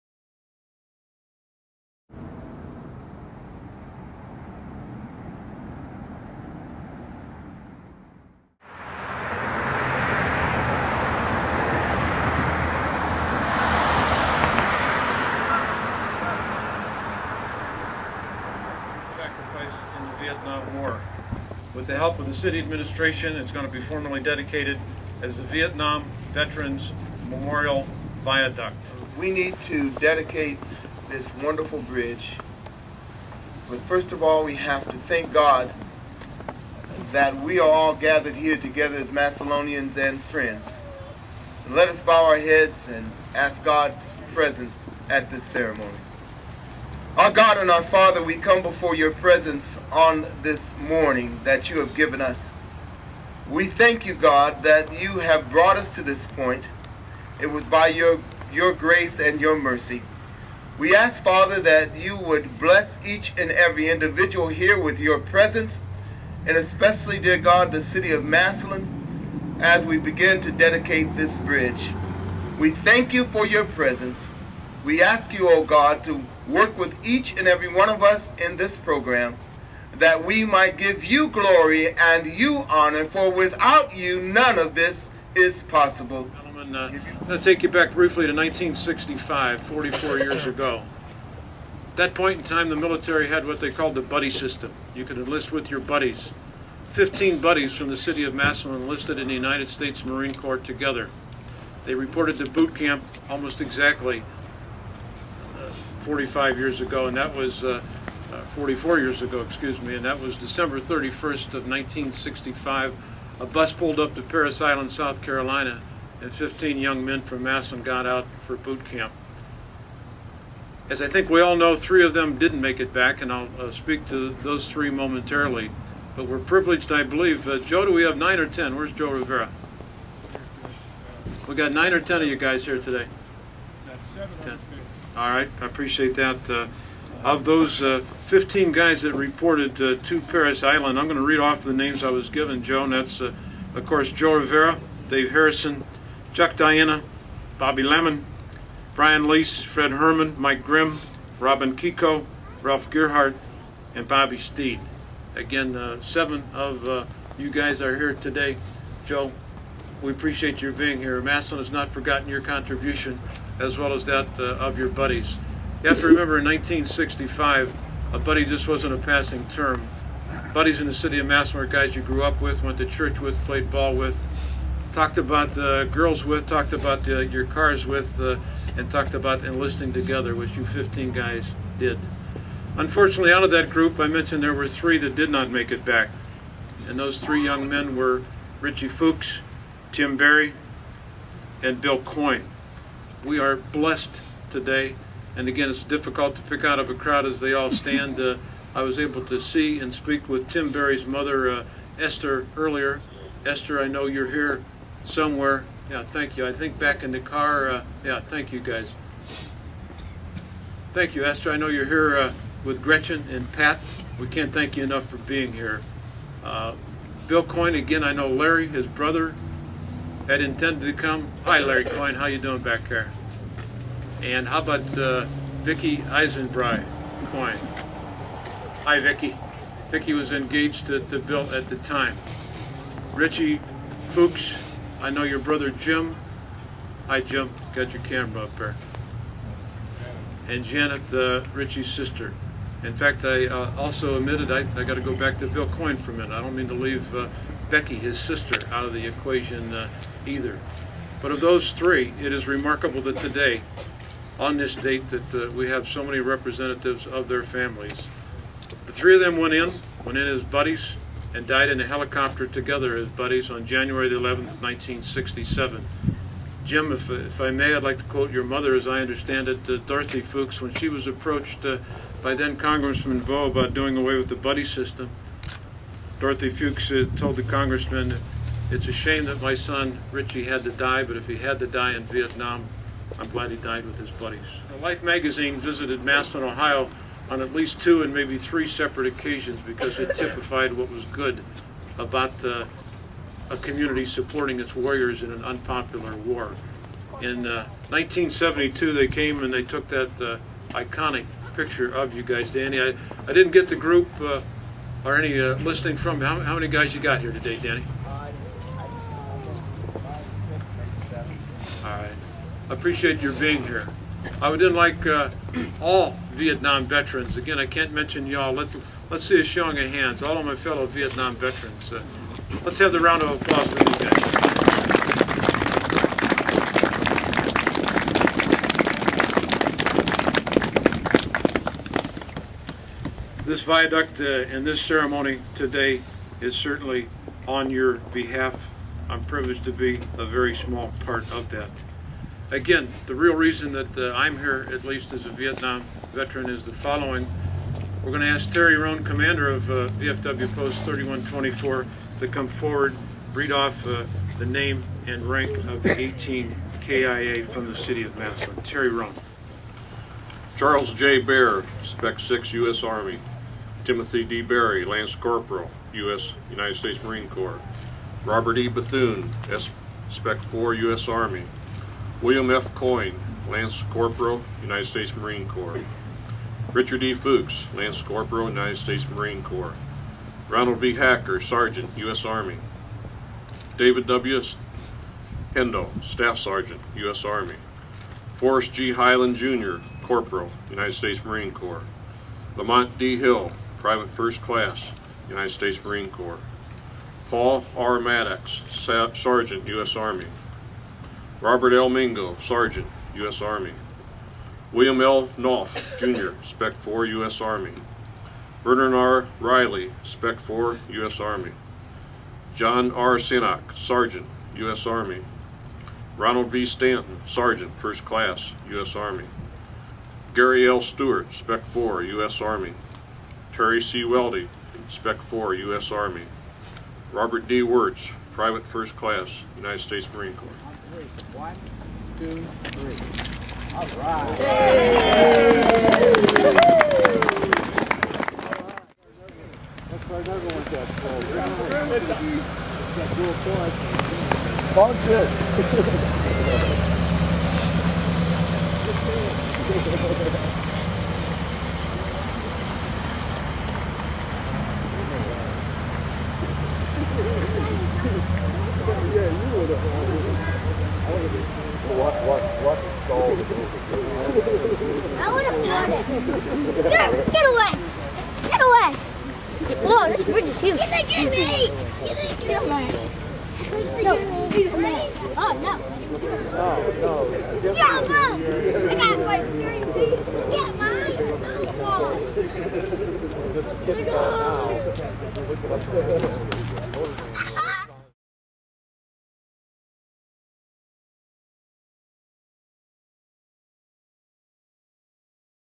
Vietnam Veterans Memorial Viaduct Dedication
The old Tremont Viaduct in Massillon, Ohio, has been replaced by the Vietnam Veterans Memorial Viaduct. The dedication took place December 19, 2009, on the east end of the new bridge.